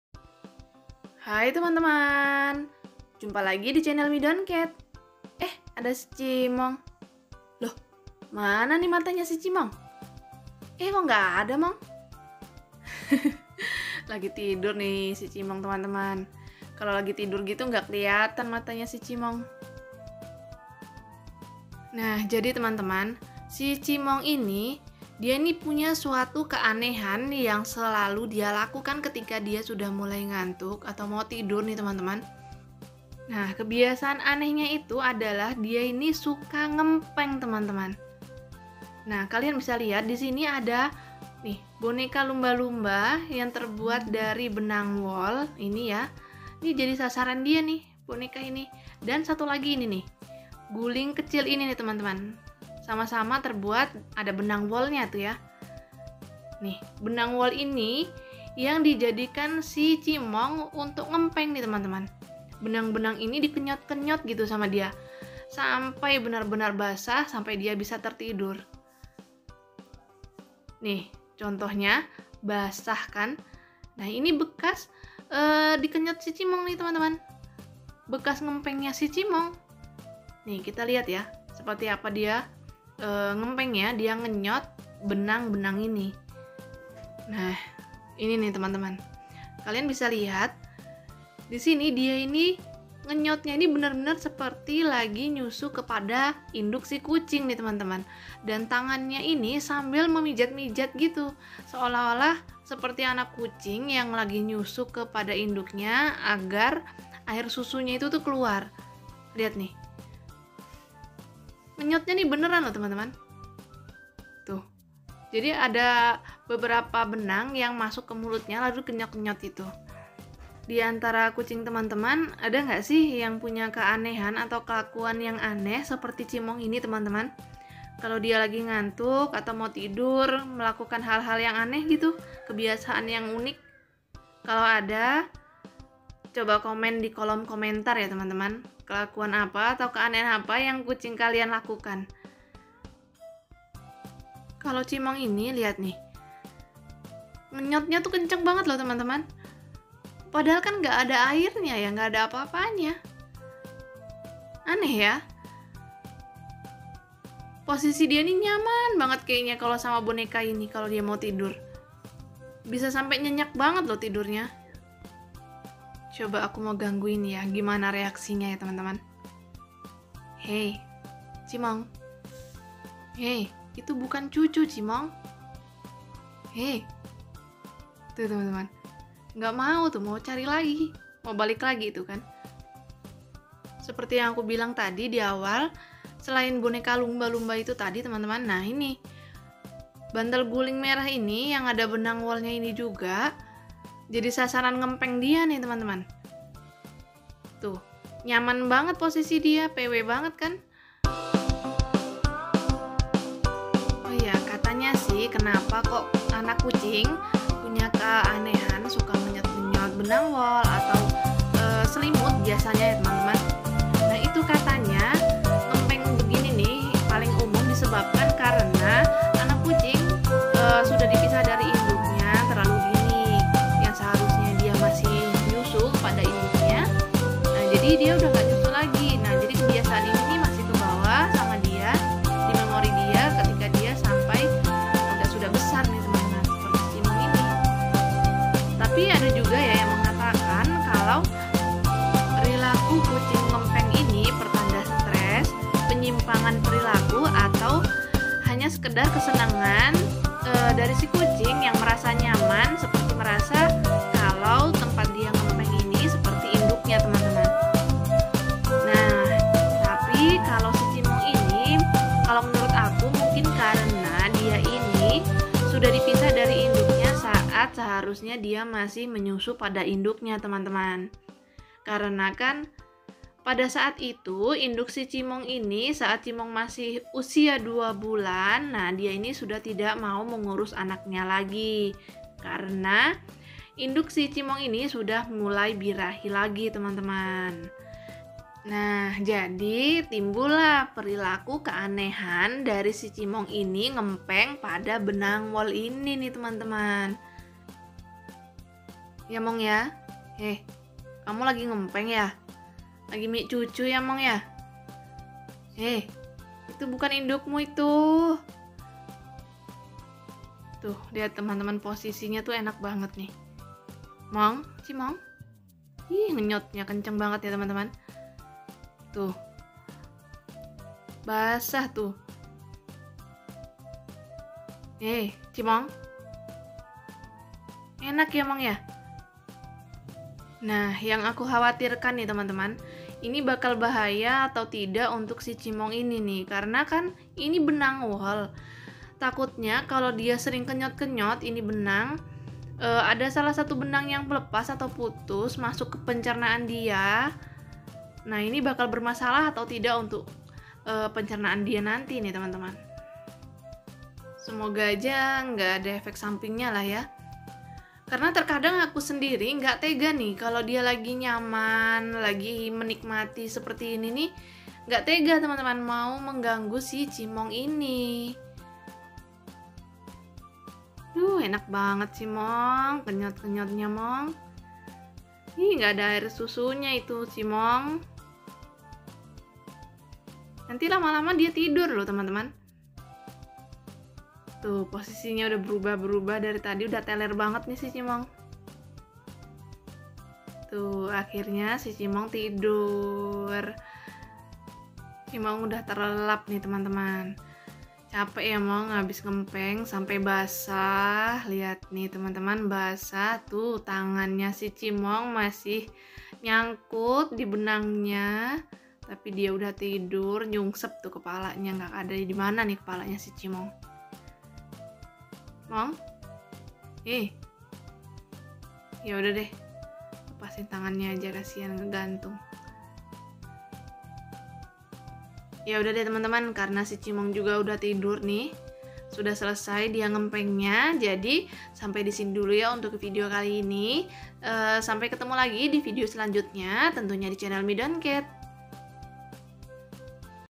KEBIASAAN ANAK KUCING SUKA NGEMPENG
Lucunya kebiasaan unik si cimong sebelum dia tidur.